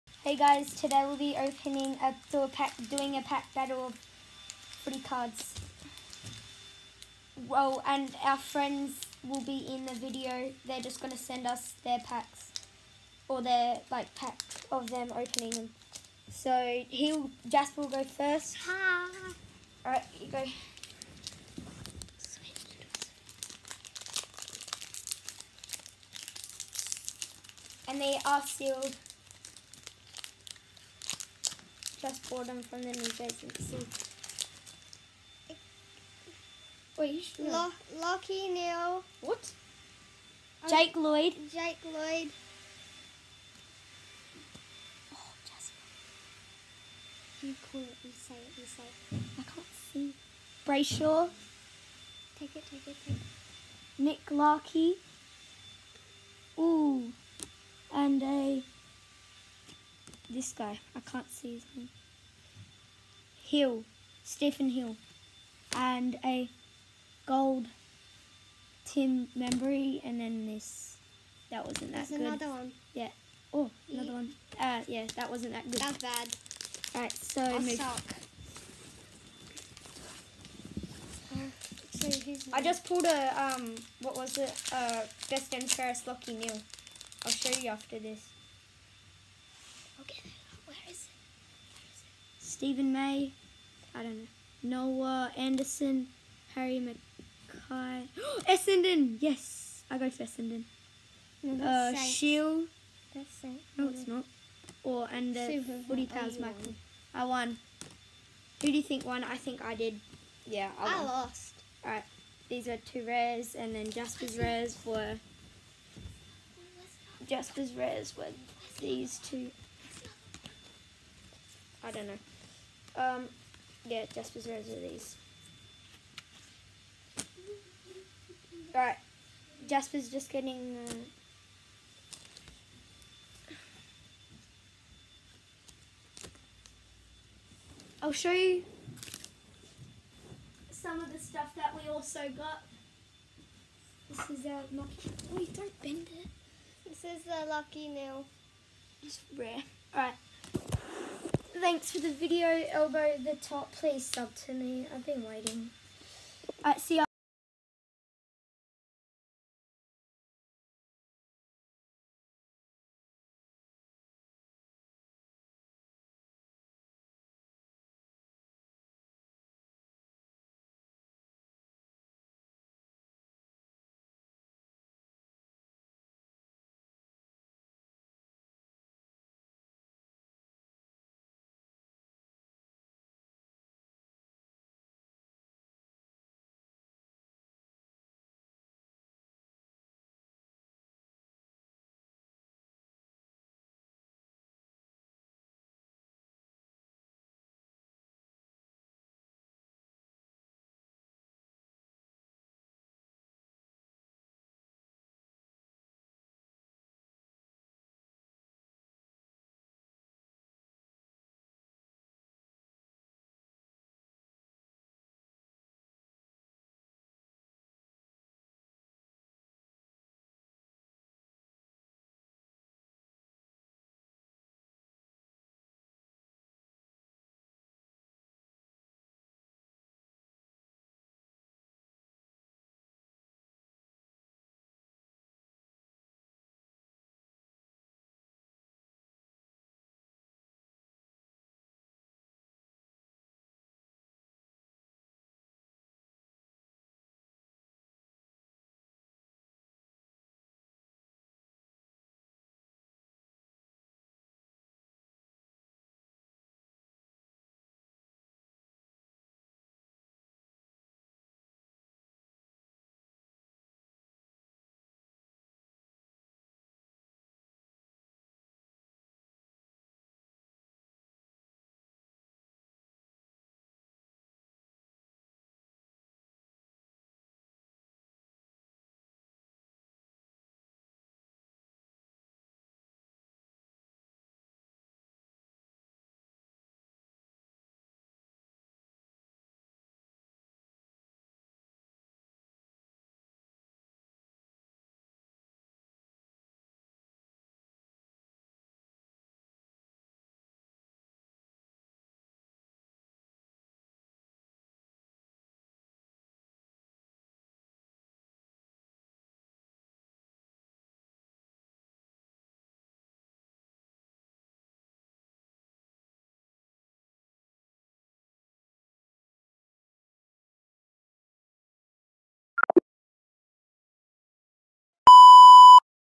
Opening some team coach footy cards with friends and also first look at the new 2021 season cards
Thank u Theo and Louis for helping